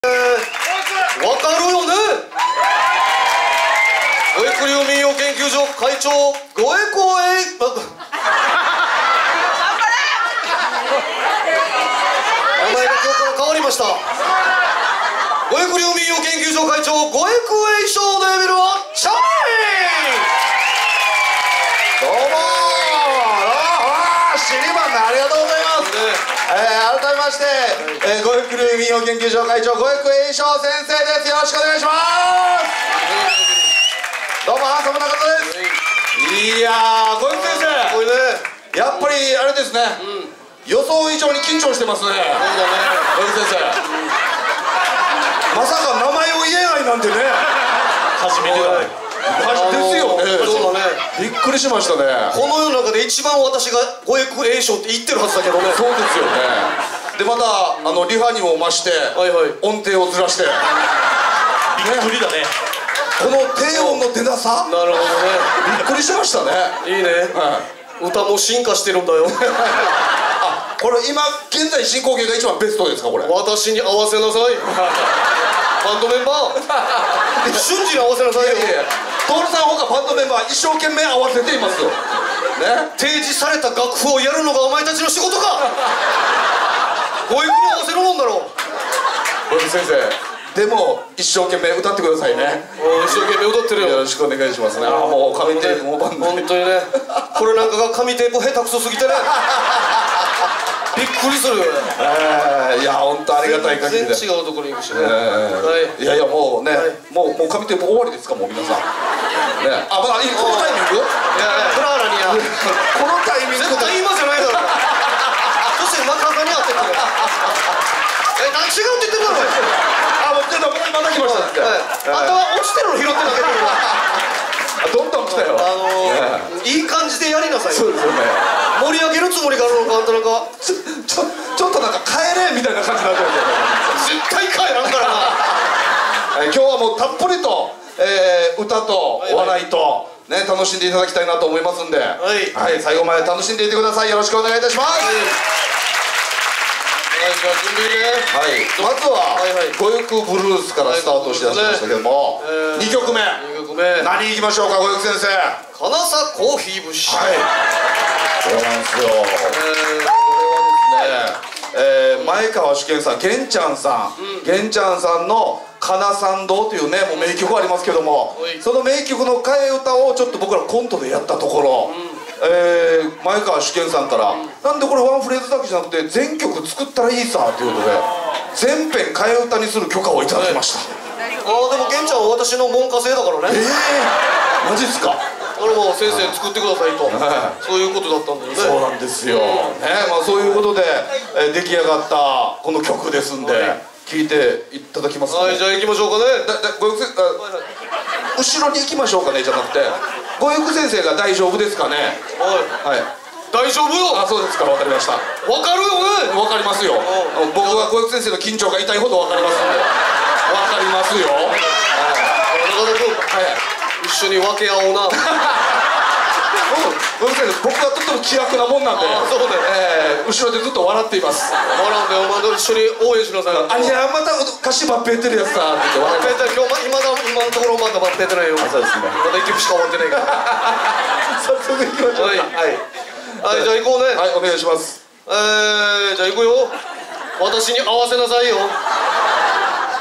【わかるよね】護得久 栄昇 MC1 報恩謝徳【護得久流民謡研究所会長】
昨年末2017/12/30に桜坂セントラルで行われましたよなは徹さんと護得久先生のツーマンLIVEでの様子です。
緊張のあまり、まさかの自己紹介で自分の名前を噛んでしまいます。